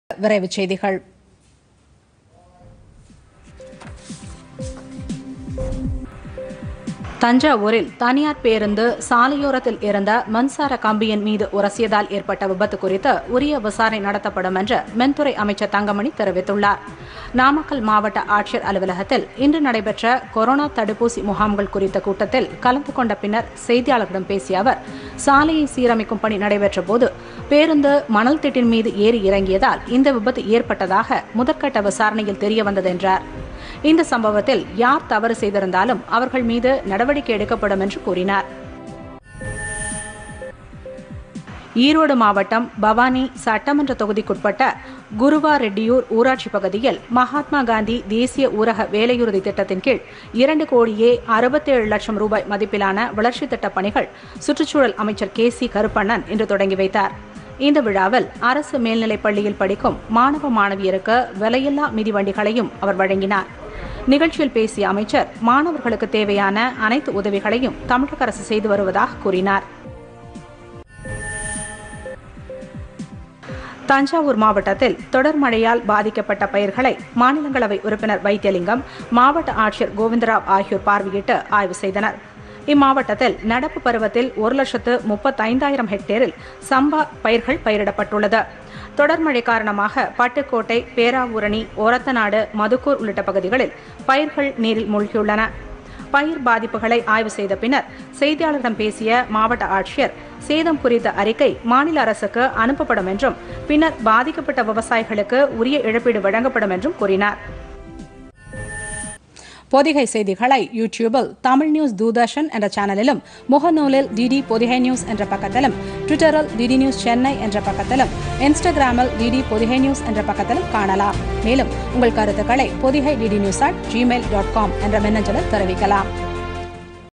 பொதிகை இரவு 7.00 மணி விரைவு செய்திகள் [13.01.2021]
1) தஞ்சாவூரில் தனியார் பேருந்து சாலையோரத்தில் இருந்த மின்சார கம்பியின்மீது உராசியதால் ஏற்பட்ட விபத்து குறித்து உரிய விசாரணை நடத்தப்படும் என்று மின்துறை அமைச்சர் தங்கமணி தெரிவித்துள்ளார். நாமக்கல் மாவட்ட ஆட்சியர் அலுவலகத்தில் இன்று நடைபெற்ற ‍கொரோனா தடுப்பூசி முகாம்கள் குறித்த கூட்டத்தில் கலந்துகொண்ட பின்னர் செய்தியாளர்களிடம் பேசிய அவர், சாலையை சீரமைக்கும் பணி நடைபெற்ற போது பேருந்து மணல் திட்டின்மீது ஏறி இறங்கியதால் இந்த விபத்து ஏற்பட்டாக முதற்கட்ட விசாரணையில் தெரியவந்தது என்றார். இந்த சம்பவத்தில் யார் தவறு செய்திருந்தாலும் அவர்கள் மீது நடவடிக்கை எடுக்கப்படும் என்று கூறினார்.
2)   ஈரோடு மாவட்டம், பவானி சட்டமன்ற தொகுதிக்குட்பட்ட குருவாரெட்டியூர் ஊராட்சி பகுதியில் மகாத்மாகாந்தி தேசிய ஊரக வேலையுறுதி திட்டத்தின்கீழ் 2 கோடியே 67 லட்ச ரூபாய் மதிப்பிலான வளர்ச்சித்திட்டப் பணிகளை சுற்றுச்சூழல் அமைச்சர் கே சி கருப்பண்ணன் இன்று தொடங்கி வைத்தார். இந்த விழாவில் அரசு மேல்நிலைப் பள்ளியில் படிக்கும் மாணவ-மாணவியருக்கு விலையில்லா மிதிவண்டிகளையும் அவர் வழங்கினார். நிகழ்ச்சியில் பேசிய அமைச்சர், மாணவர்களுக்கு தேவையான அனைத்து உதவிகளையும், தமிழக அரசு செய்துவருவதாக கூறினார்.
3)  தூத்துக்குடி மாவட்டத்தில் பெய்துவரும் கனமழை காரணமாக தாமிரபரணி ஆற்றில் வெள்ளம் கரைபுரண்டு ஓடுகிறது. இதனால், ஆற்றின் கரையோரம் வசிக்கும் மக்கள் பாதுகாப்பான இடங்களுக்கு செல்லுமாறு அறிவுறுத்தப்பட்டுள்ளனர். தாழ்வான பகுதிகளில் வசிக்கும் மக்களுக்காக மாவட்டத்தில், 7 இடங்களில் தற்காலிக தங்கும் முகாம்கள் அமைக்கப்பட்டுள்ளதாக மாவட்ட ஆட்சியர் செந்தில் ராஜ் கூறினார். முன்னதாக அவர், தாமிரபரணி ஆற்றுப்படுகை உள்ளிட்ட வெள்ளம் சூழ்ந்துள்ள பகுதிகளை அதிகாரிகளுடன் சென்று பார்வையிட்டார்.
4) தஞ்சாவூர் மாவட்டத்தில் தொடர்மழையால் பாதிக்கப்பட்ட பயிர்களை மாநிலங்களவை உறுப்பினர் வைத்திலிங்கம், மாவட்ட ஆட்சியர் கோவிந்த ராவ் ஆகியோர் பார்வையிட்டு ஆய்வு செய்தனர். இம்மாவட்டத்தில் நடப்பு பருவத்தில் ஒரு லட்சத்து 35 ஆயிரம் ஹெக்டேரில் சம்பா தாளடி பயிர்கள் பயிரிடப்பட்டுள்ளது. தொடர் மழை காரணமாக பட்டுக்கோட்டை, பேராவூரணி, ஒரத்தநாடு, மதுக்கூர் உள்ளிட்ட பகுதிகளில் பயிர்கள் நீரில் மூழ்கியுள்ளன. பயிர் பாதிப்புகளை ஆய்வு செய்த பின்னர், செய்தியாளர்களிடம் பேசிய மாவட்ட ஆட்சியர், சேதம் குறித்த அறிக்கை மாநில அரசுக்கு அனுப்பப்படும் என்றும் பின்னர் பாதிக்கப்பட்ட விவசாயிகளுக்கு உரிய இழப்பீடு வழங்கப்படும் என்று கூறினார்.
5) திருநெல்வேலி மாவட்டத்தில் பெய்த கனமழை காரணமாக தாமிரபரணி ஆற்றில் ஏற்பட்ட வெள்ளப்‍பெருக்கால், நீரேற்று கிணறுகள், நீரேற்று நிலையங்கள் அனைத்தும் தண்ணீரில் மூழ்கியுள்ளதால்,  திருநெல்வேலி மாநகராட்சியின் அனைத்து வார்டுகளுக்கும் இன்று முதல் வரும் 16 ஆம் தேதி வரை குடிநீர் விநியோகம் நிறுத்தப்பட்டுள்ளதாக மாநகராட்சி ஆணையர் கண்ணன் தெரிவித்துள்ளார். தொடர்மழை காரணமாக பொதுமக்கள் குடிநீரை சிக்கனமாக பயன்படுத்த வேண்டுமென்றும் கொதிக்க வைத்து அருந்துமாறும் அவர் கேட்டுக்கொண்டுள்ளார்.
6) சார்ஜாவிலிருந்து விமானத்தில் சென்னை வந்த இருவரிடமிருந்து சுமார் ஒரு கிலோ 420 கிராம் கடத்தல் தங்கம் பறிமுதல் செய்யப்பட்டது. இருவரது பைகளில் சுங்கத்துறையினர் தொடர்ந்து சோதனையிட்டபோது வெளிநாட்டு சிகரெட்டுகளும், 11 ‍ஐபோன்களும், 8 பழைய லேப்டாப்களும், 8 மதுபாட்டில்களும் பறிமுதல் செய்யப்பட்டன. தங்கம் மற்றும் இப்பொருட்களின் மதிப்பு 85 லட்ச ரூபாய்
 என்றும் அந்த இரண்டு பேரும் கைது செய்யப்பட்டு விசாரிக்கப்பட்டுவருவதாகவும் விமானநிலைய சுங்கத்துறை ஆணையர் தெரிவித்துள்ளார்.
7) கொரோனா தொற்றிலிருந்து பாதுகாத்துக் கொள்வது தொடர்பாக விழிப்புணர்வு வீடியோ வாகனத்தை சேலத்தில் மாவட்ட ஆட்சியர் ராமன் இன்று தொடங்கி வைத்தார். மக்கள் கூடும் இடங்களில் இந்த வாகனங்கள் சென்று நோய் குறித்து விழிப்புணர்வை ஏற்படுத்தும் நோய்த்தொற்று ஏற்பட்டால் மேற்கொள்ளப்படும் சிகிச்சை முறைகளும் இதில் விளக்கப்படும். சின்னத்திரை நடிகர்கள் நடித்த கொரோனா விழிப்புணர்வு வீடியோவும் இந்த வாகனங்களில் இடம்பெறும்.
8)   விழுப்புரத்தில் தமிழர்களின் பாரம்பரிய விளையாட்டுக்களில் ஒன்றான மல்லர் விளையாட்டு வீரர்கள் ஒன்றுகூடி சமத்துவ பொங்கலிட்டனர். நகராட்சி பூங்காவில் நடைபெற்ற இந்த நிகழ்ச்சியில் ஏராளமான மல்லர் கம்பம் விளையாட்டு வீரர்கள் கலந்துகொண்டனர். பொங்கல் பொங்கிவரும் போது‍ அனைவரும் பொங்களோ ‍பொங்கல் என்று கூறி ஒருவருக்கொருவர் பொங்கல் வாழ்த்துக்களை தெரிவித்துக்‍கொண்டனர்.
9) ஒகேநக்கல் சுற்றுலா தளத்தில் தமிழ்நாடு சுற்றுலாத் துறை சார்பில், சமத்துவ பொங்கல் விழா இன்று நடைபெற்றது. இதை முன்னிட்டு கரகாட்டம், ஒயிலாட்டம் உள்ளிட்ட தமிழர்களின் பாரம்பரிய கலைநிகழ்ச்சிககை்கும், இசை நிகழ்ச்சிகளுக்கும் ஏற்பாடு செய்யப்பட்டிருந்தது. கலைநிகழ்ச்சிகளை சுற்றுலா பயணிகள் மிகுந்த ஆர்வத்துடன் கண்டுகளித்தனர். தமிழகம் மட்டுமின்றி, ஆந்திரா, கர்நாடகம் உள்ளிட்ட மாநிலங்களை சேர்ந்த சுற்றுலா பயணிகளும் இந்தப் பொங்கல் விழாவில் உற்சாகமாக கலந்துகொண்டனர்.
10) கர்நாடகாவில் முதலமைச்சர் எடியூரப்பா தலைமையிலான அமைச்சரவை இன்று விரிவாக்கம் செய்யப்பட்டுள்ளது. அமைச்சரவையில் புதிதாக 7 பேர்  சேர்க்கப்பட்டுள்ளனர்.